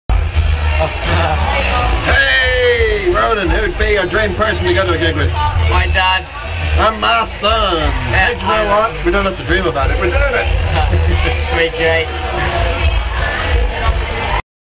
The Milk - Who would be your dream person to go to a gig with?
O2 asked fans at O2 Academy Liverpool: "Who would be your dream person to go to a gig with?"